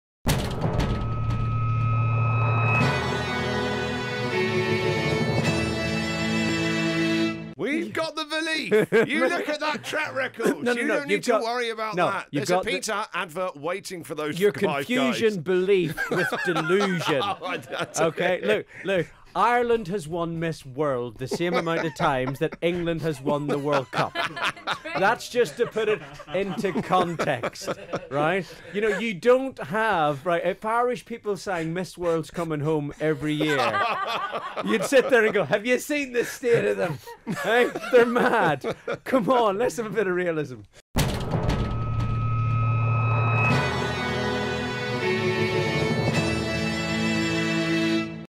Patrick Kielty On England In The World Cup
Comedian Patrick Kielty was our guest and he had his own thoughts on the England vs Germany game this weekend.